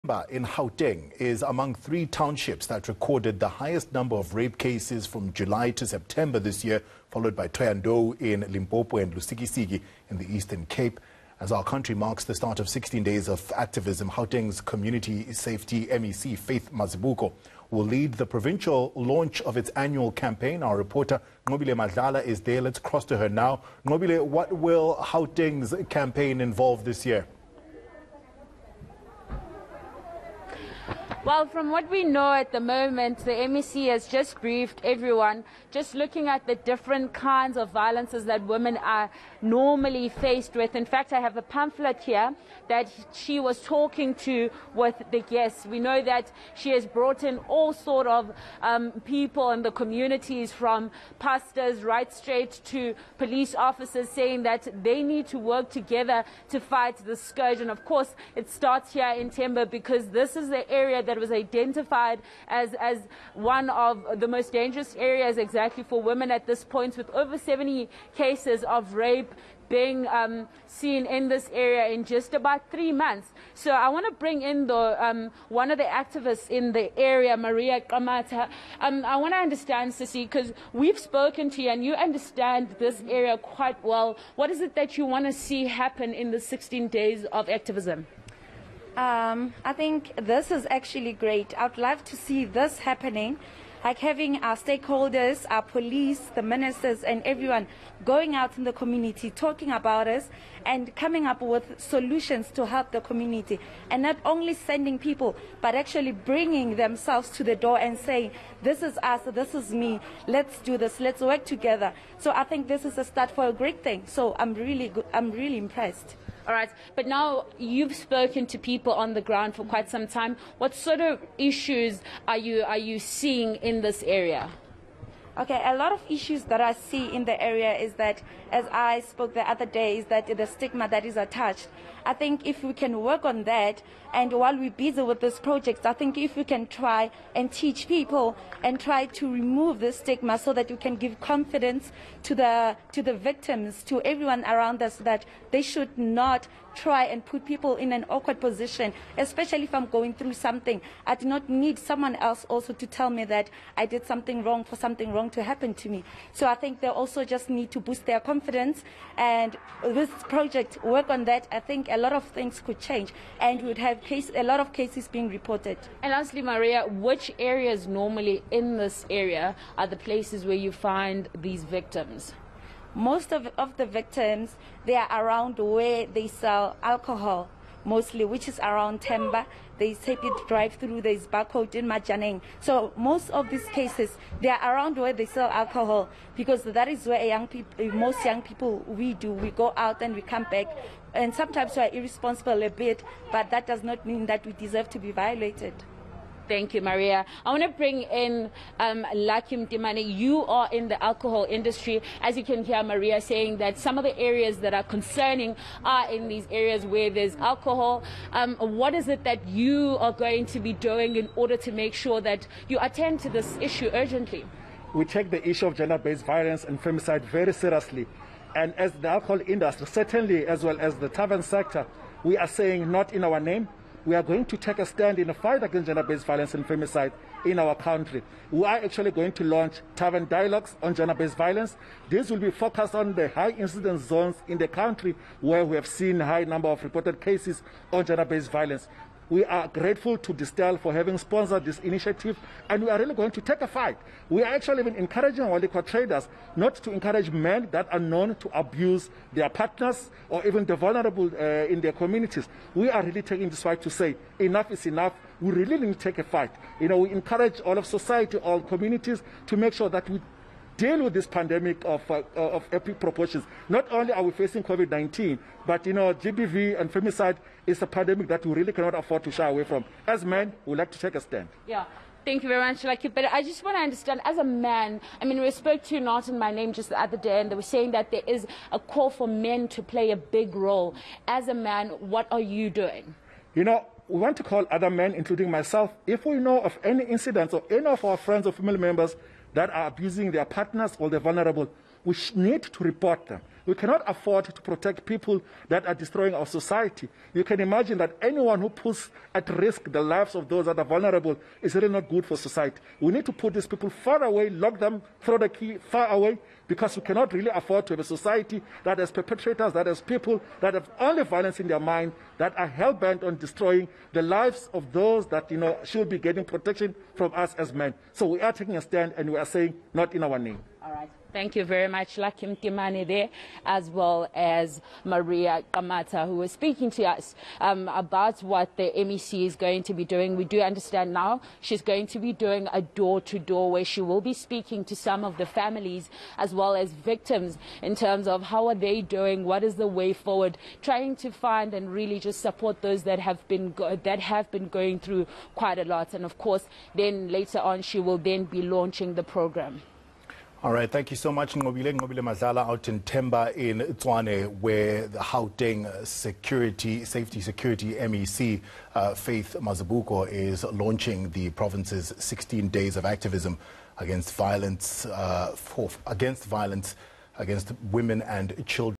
Government launches 16 Days campaign
Temba in Gauteng is among three townships that recorded the highest number of rape cases from July to September this year, followed by Thohoyandou in Limpopo and Lusikisiki in the Eastern Cape.Gauteng's Community Safety MEC, Faith Mazibuko will lead the provincial launch of its annual campaign. eNCA's Nqobile Madlala has more. Courtesy